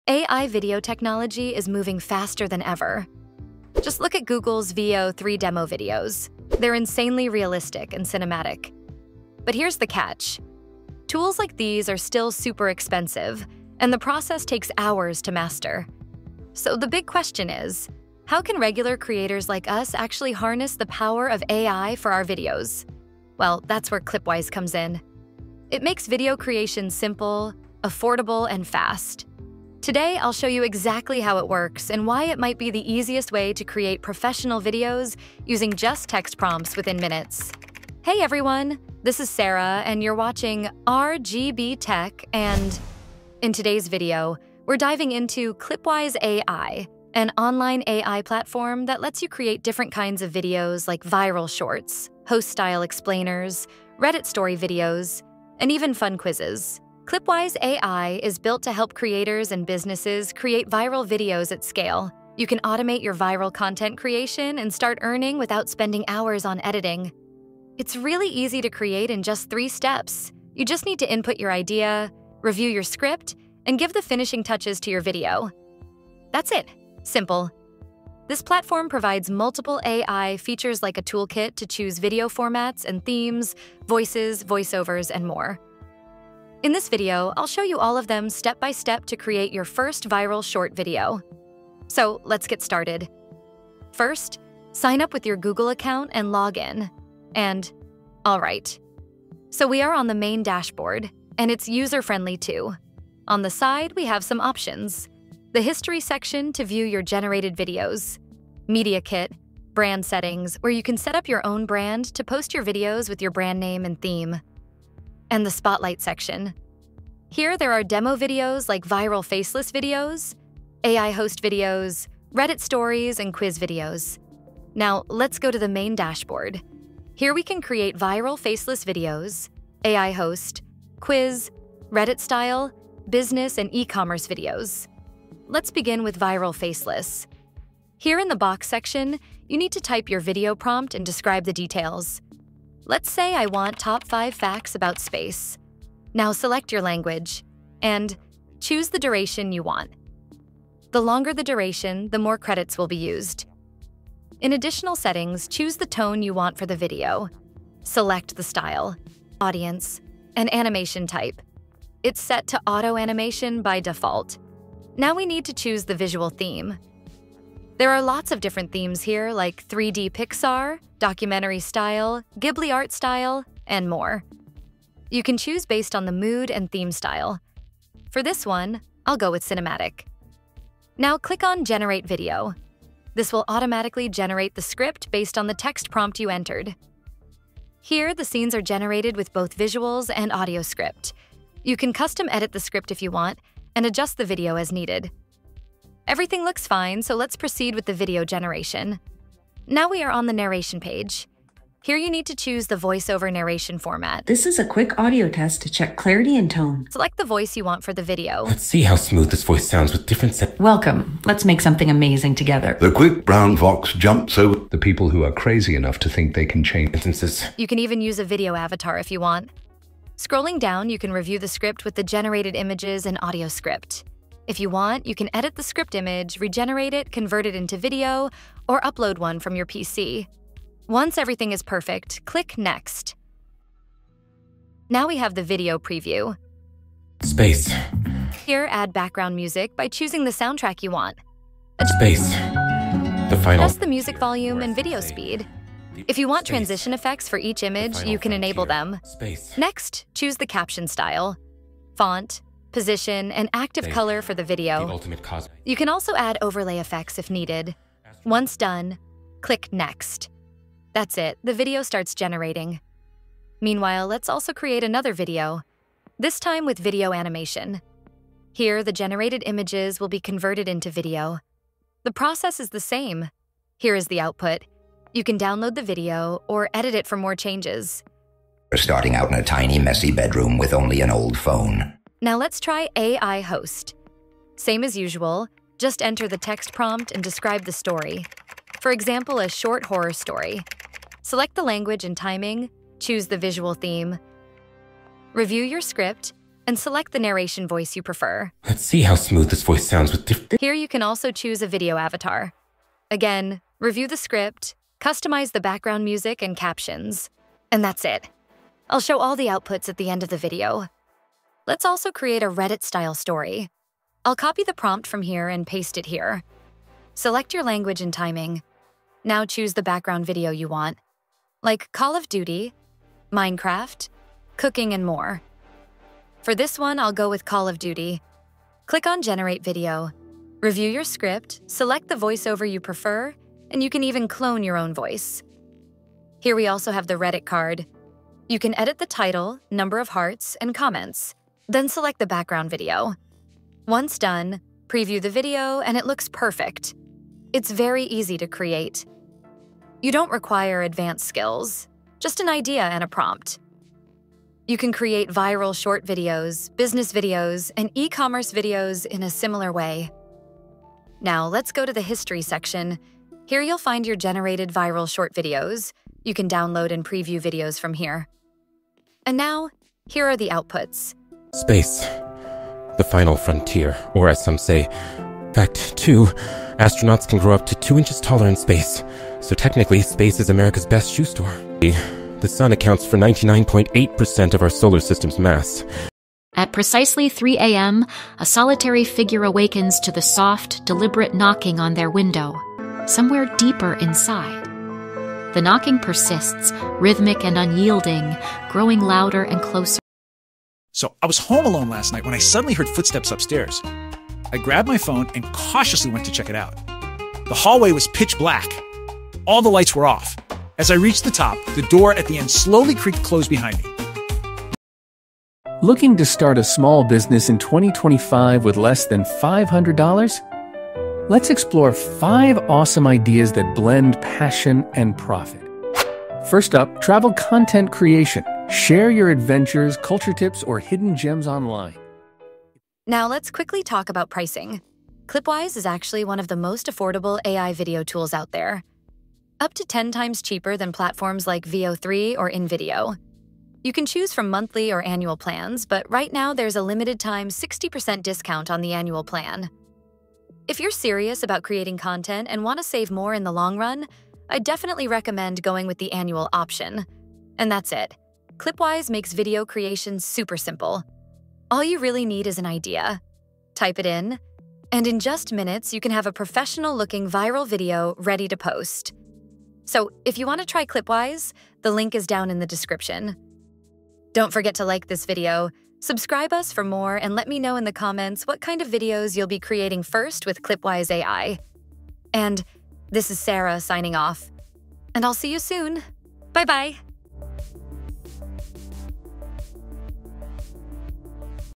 How to Create Viral YouTube Shorts with Clipwise AI (No Editing Needed)
🚀 Clipwise AI is the easiest way to create viral videos in 2025. Whether you want faceless shorts, AI hosts, Reddit-style stories, business videos, or quizzes, Clipwise makes it possible in just 3 simple steps—no editing skills required!

In this video, I’ll give you a full Clipwise AI tutorial + walkthrough, showing how to:
✅ Create viral faceless videos
✅ Generate AI host videos with avatars
✅ Make Reddit story videos with gameplay backgrounds
✅ Build quiz & business videos
✅ Customize everything (voices, captions, themes, effects & more)

Time Stamps:
0:00 Introduction
0:42 Clipwise webpage
1:37 Main Dashboard
2:15 Create Viral Faceless Tutorial
4:42 AI Avatar Host
5:11 Reddit Style Video
6:00 Output Results
7:12 Pricing and Discount
7:42 Outro